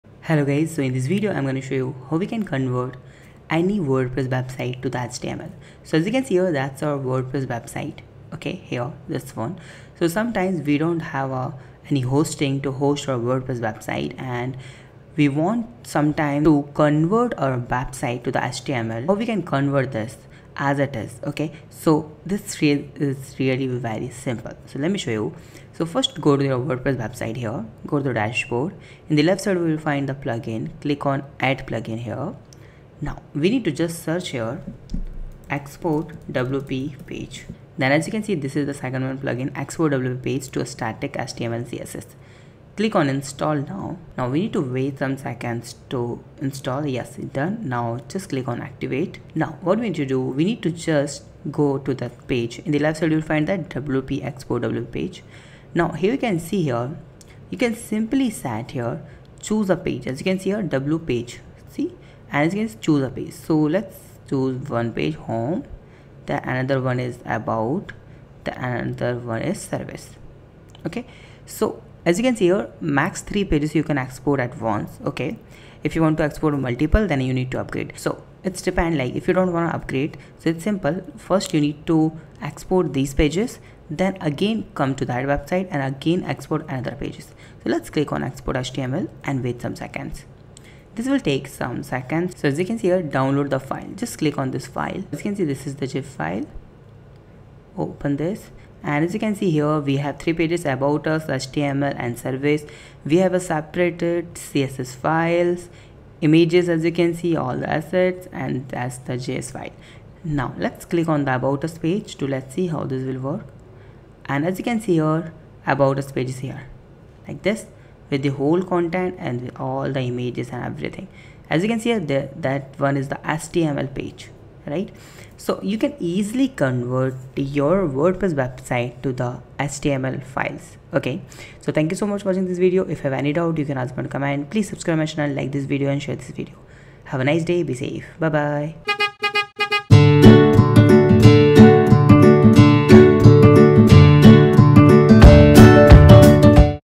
How To Convert Any Wordpress Website To HTML Website
(Working)

Export WP page to static HTML/CSS plugin is s static html site or page generator thats help you to convert your wordpress site or pages to static html website and you can host it on your server, from a static html hosting provider or from a cdn.

It will magically improves your site performance and reduce any security risk because it removes database connection from your server.

Features
Export html page in one click.
Lite weight version.
Easy layout.